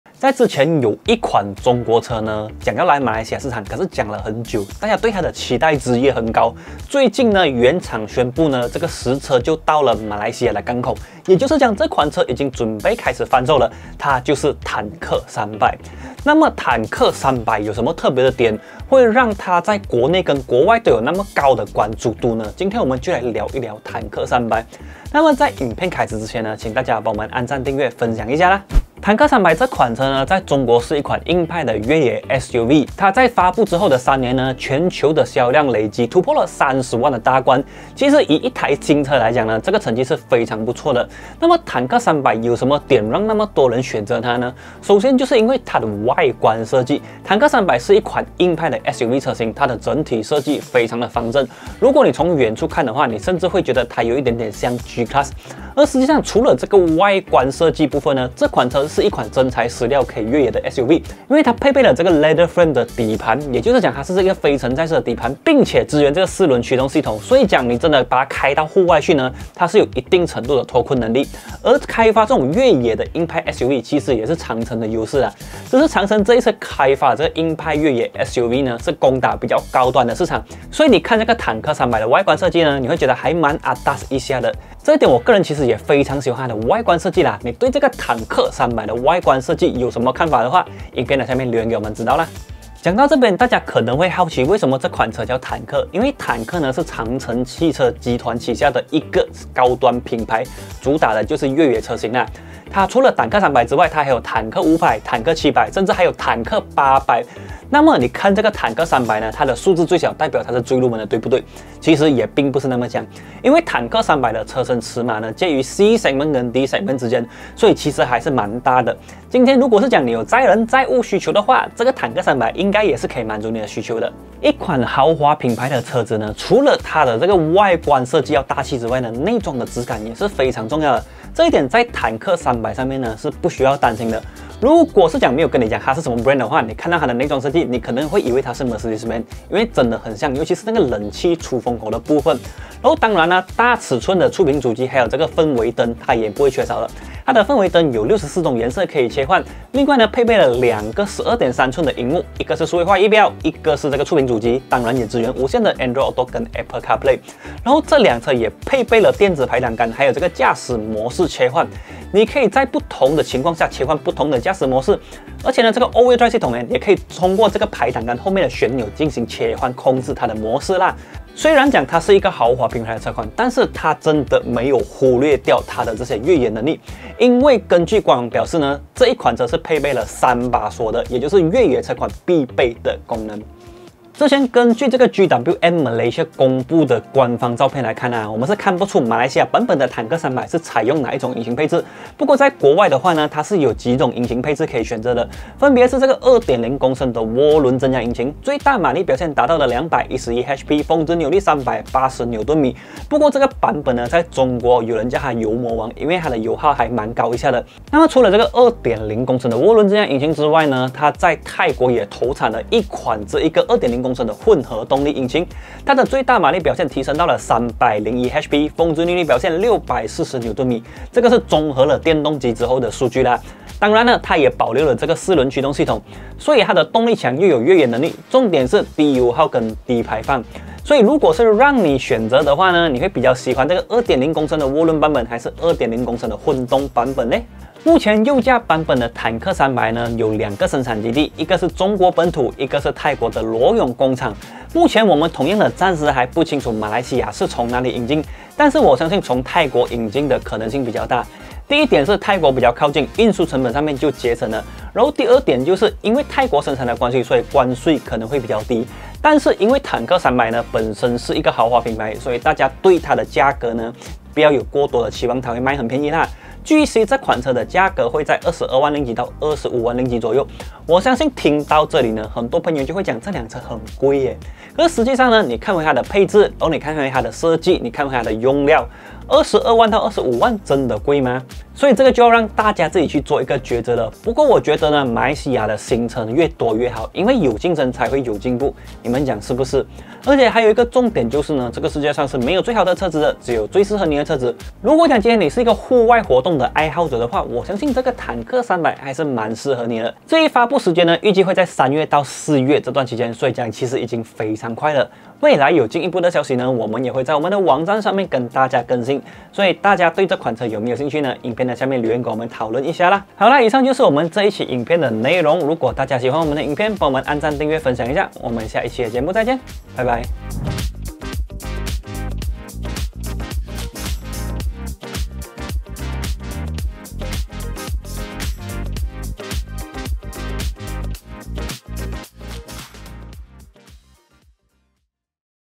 GWM Tank 300 最快三月登陆大马：本地售价RM 250,000起跳你会买吗？（汽车咖啡馆）｜automachi.com 马来西亚试车频道
GWM Tank 300 自发布以来就一直备受关注，相信本地也有不少朋友在期待这款硬派越野 SUV 什么时候会登陆我国，早前 GWM Malaysia 就有宣布坦克 300 已经来到了大马港口，因此相信新车最快 3/4 月就会正式发布，如果豪华越野 SUV 开价 RM 250,000 起跳，各位老板们会给它一个机会吗？